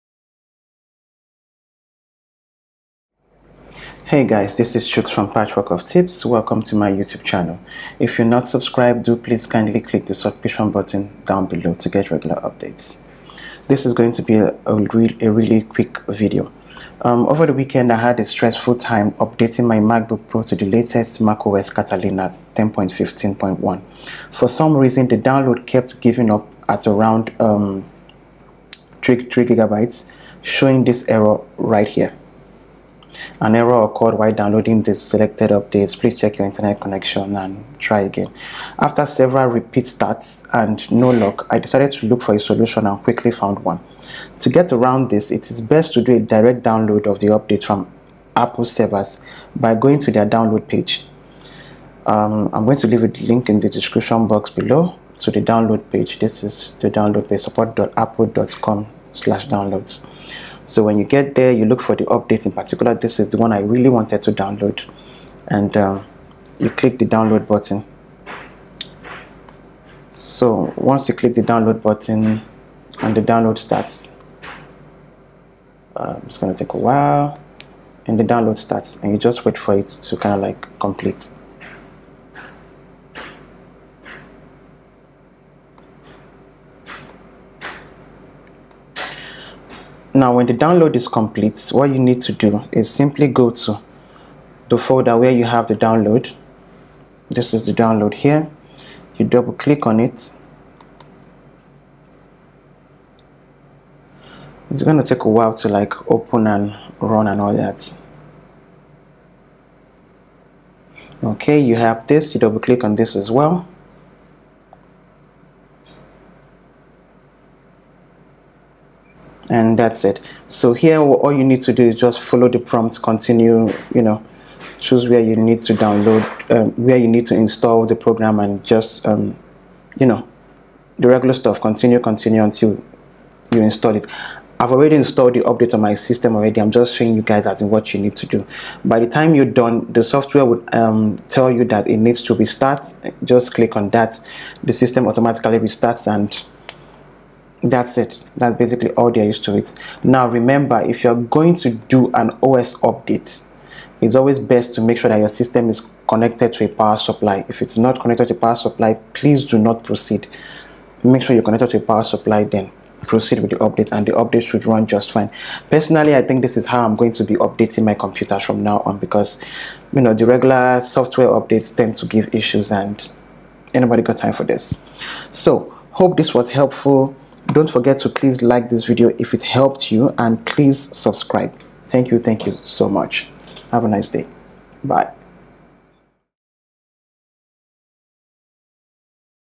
How To Fix MacOS Software Updates Download Failures
Download Failed:  An error occurred while downloading the selected updates. Please check your internet connection and try again.

This video will show you how to quickly bypass the error above on your Apple Mac, MacBook as the case may be.

Video Shoot & Editing Tools